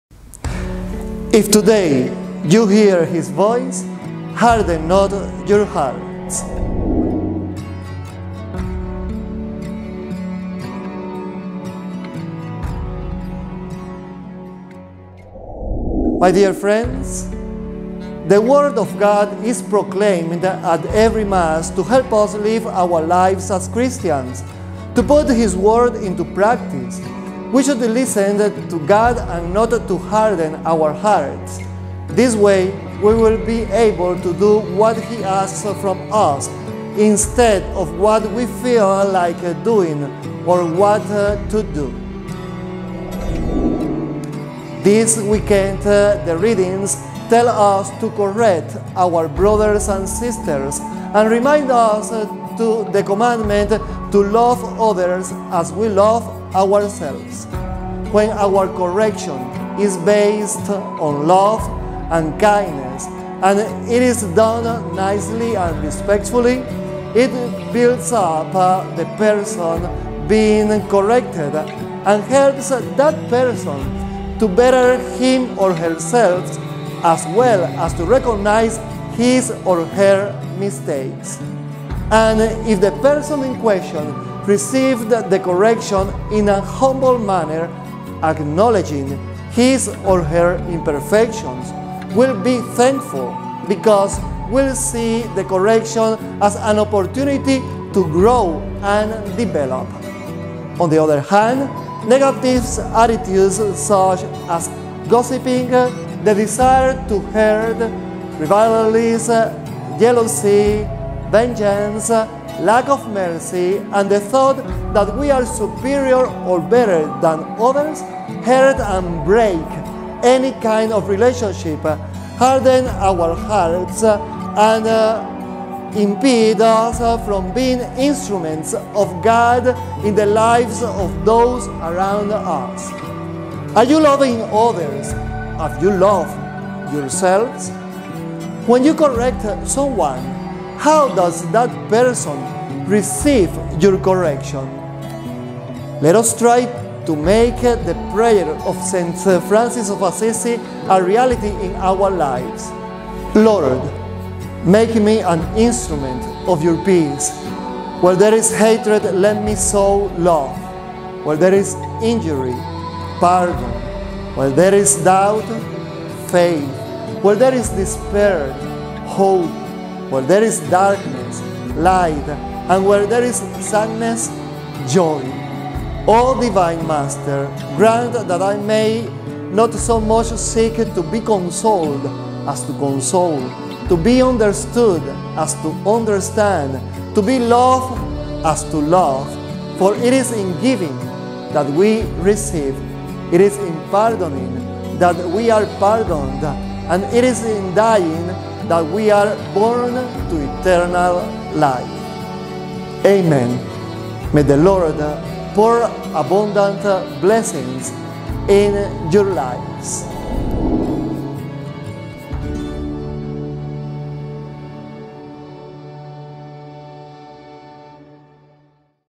CONNECT WITH YOUR FAITH - 23rd Week in Ordinary Time
Let us connect with our faith, and with each other by reflecting on Fr. Tomas' weekly message.